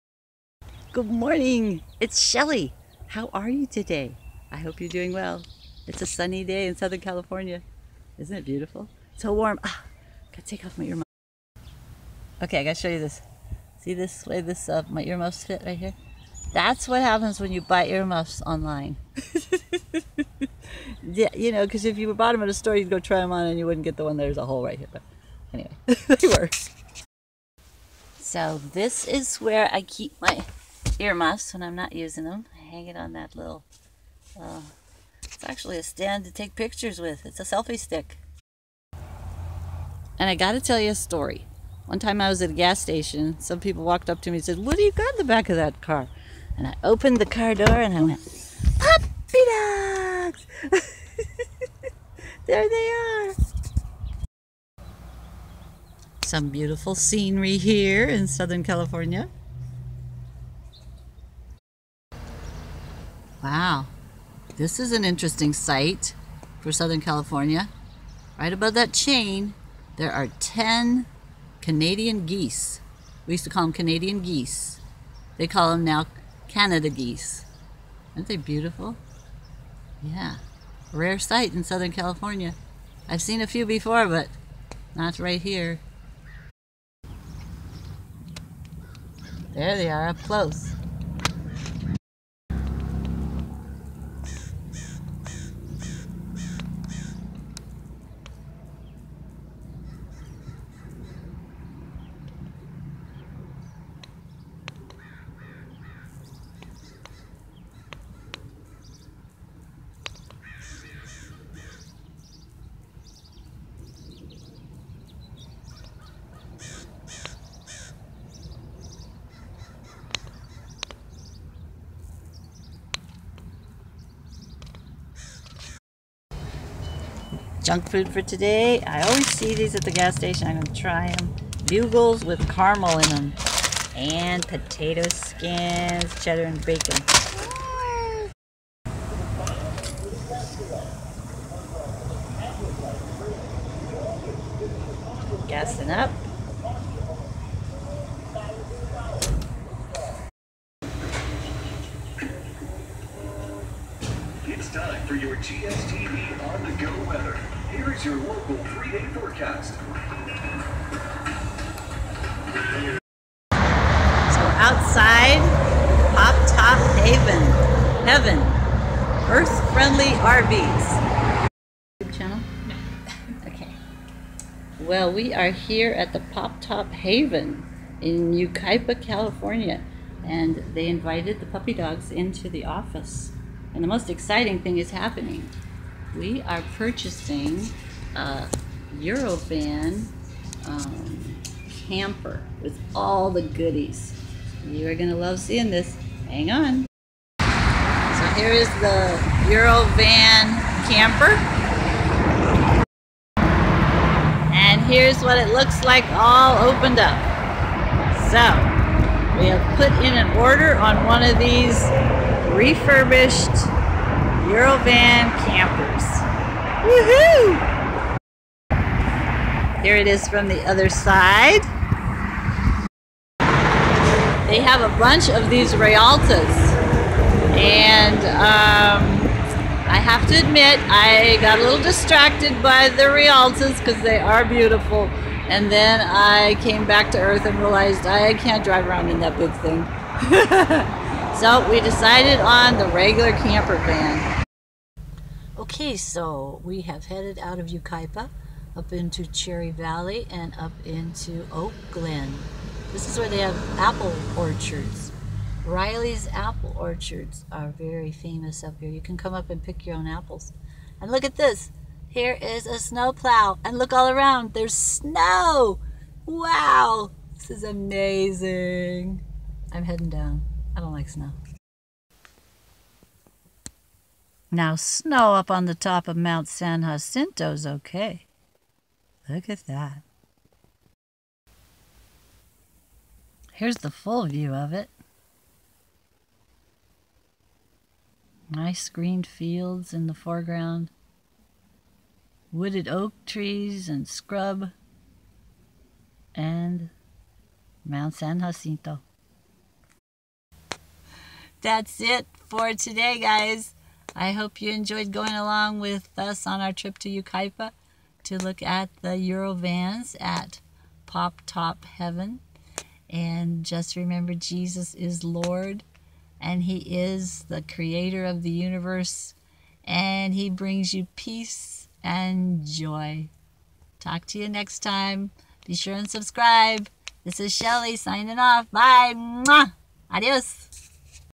Geese and Pop Top Heaven in Yucaipa - Feb 2019
We headed out to Yucaipa to look at Eurovan Campers and saw some Canadian Geese and some beautiful scenery.